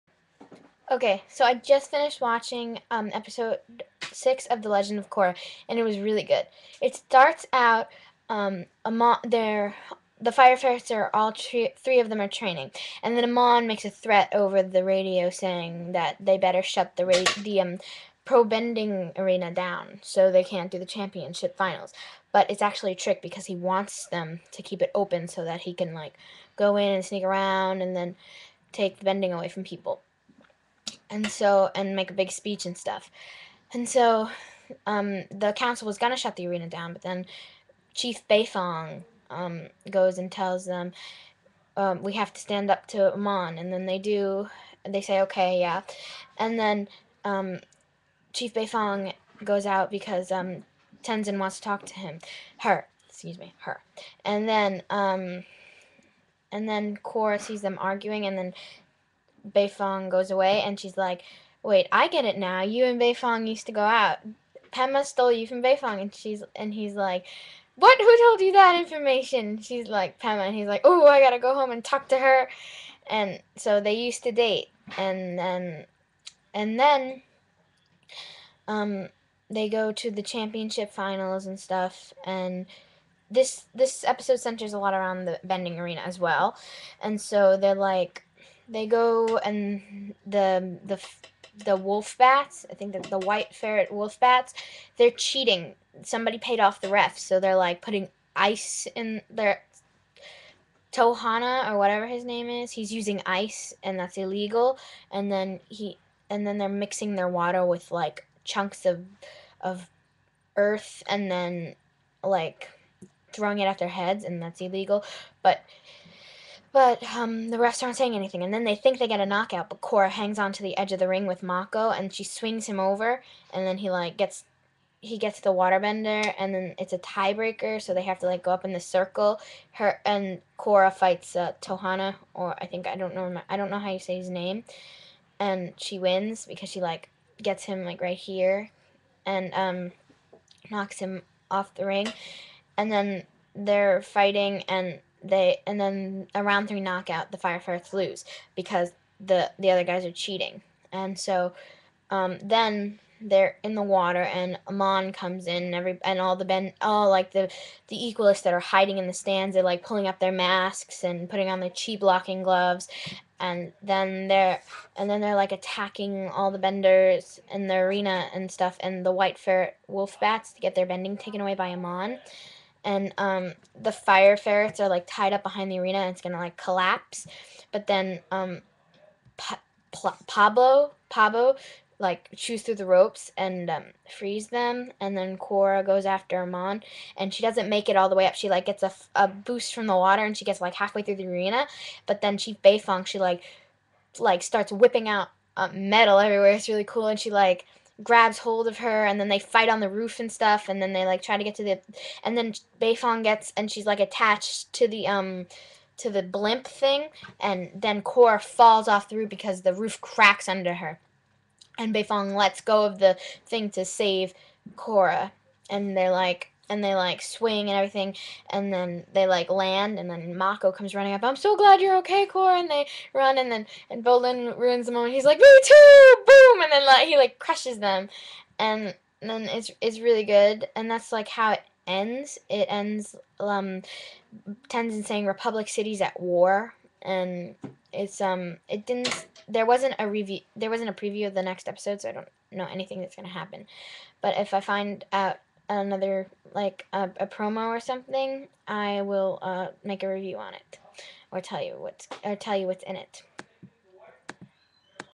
legend of korrra episode 6 review
review and recap of the legend of korra episode 6